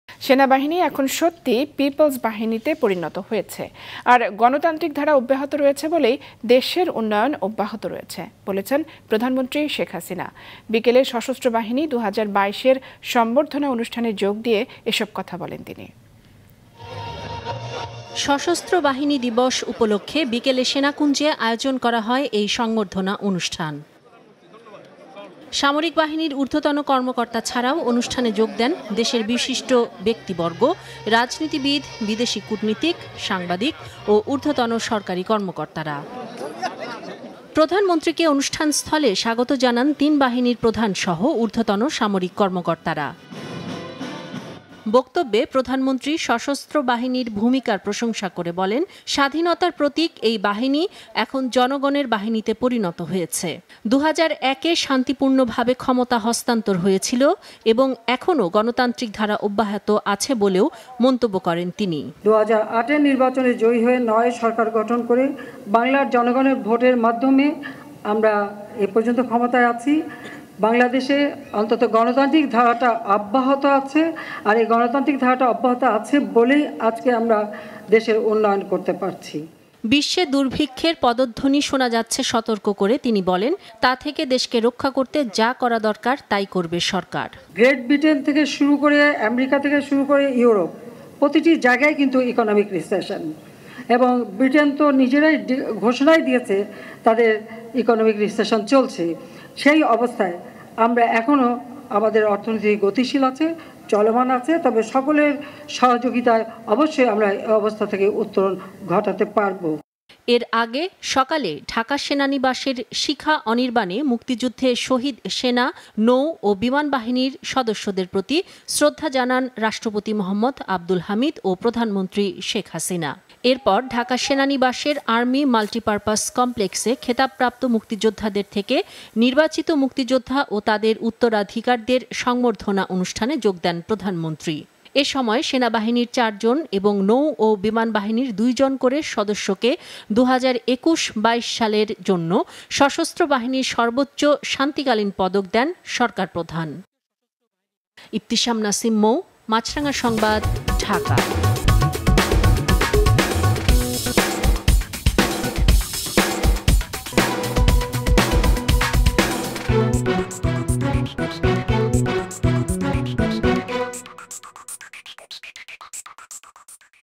সেনাবাহিনী এখন জনগণের বাহিনী | Maasranga News
সেনাবাহিনী এখন সত্যিই পিপলস বাহিনীতে পরিণত হয়েছে এবং গণতান্ত্রিক ধারা অব্যাহত আছে বলেই দেশের উন্নয়ন অব্যাহত আছে-সশস্ত্র বাহিনী দিবস ২০২২ এ প্রধানমন্ত্রী শেখ হাসিনা।

2 Bir Uttam Ziaur Rahman Road (New Airport Road)